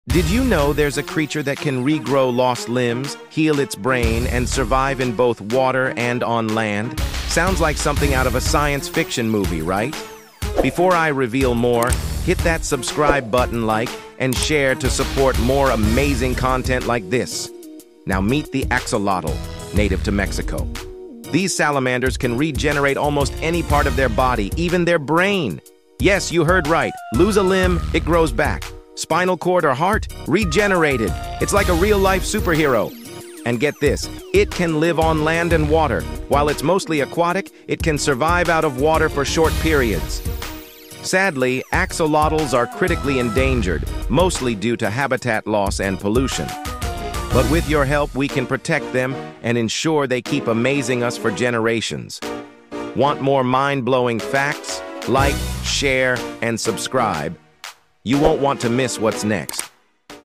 Axolotl: The Superhero Salamander That Regrows Limbs & Brain! | Mind-Blowing Facts
Did you know there’s a creature that can regrow lost limbs, heal its brain, and survive in both water and on land? Meet the axolotl, a real-life superhero that can regenerate its body parts—including its spinal cord and heart! Native to Mexico and critically endangered, these amazing salamanders are unlike anything you’ve ever seen. Watch to discover more mind-blowing facts about axolotls and learn how you can help protect them!